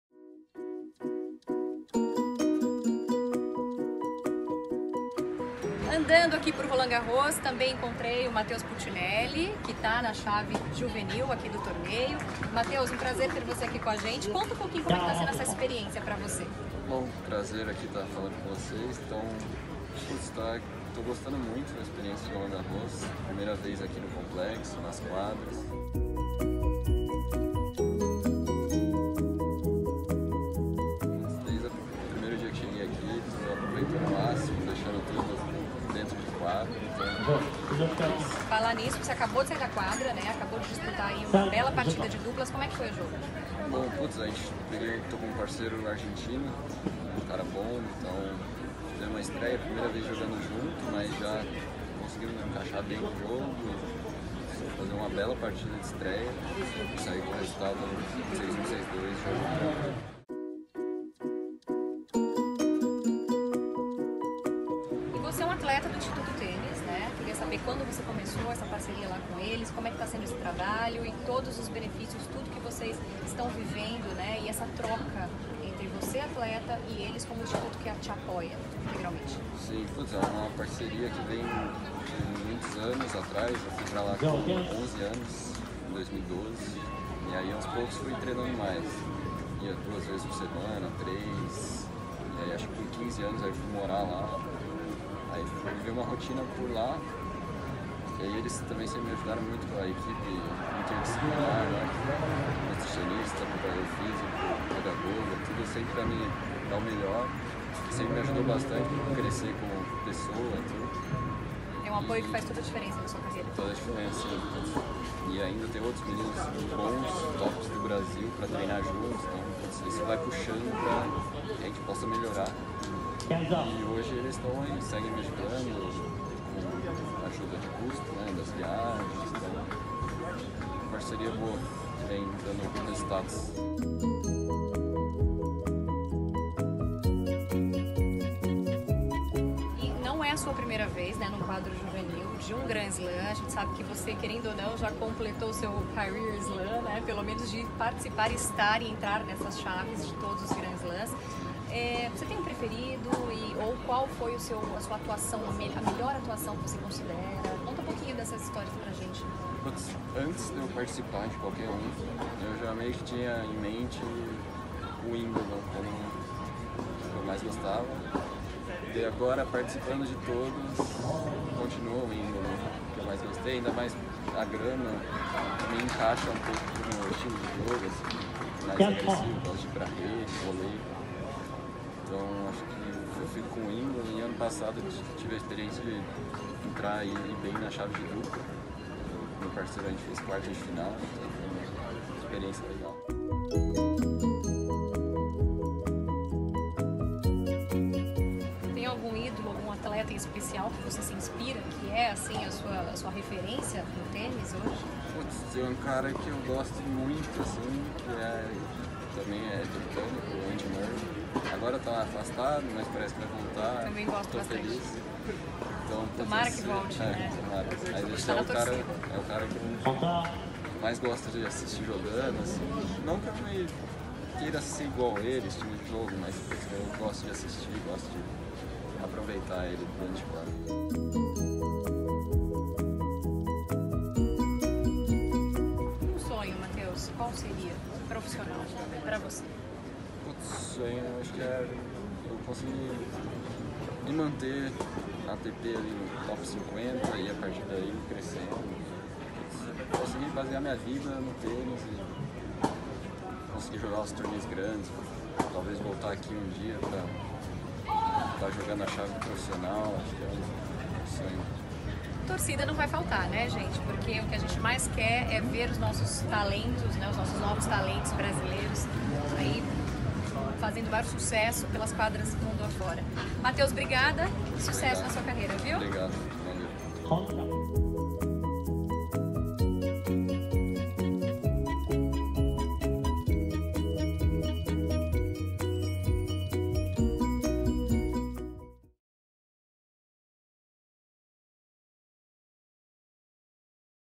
Matheus Pucinelli, campeão juvenil de duplas em Roland Garros
Antes de conquistar o título juvenil nas duplas, o Tennis Report bateu um papo com Matheus Pucinelli sobre a experiência em Roland Garros, o apoio que recebe do Instituto Tênis, sua referência no esporte e planos para o futuro. O paulista levantou o trofeu ao lado do parceiro argentino Thiago Tirante e repetiu o feito de Guga que, em 1994, foi campeão em Paris na mesma categoria, antes do tricampeonato em simples.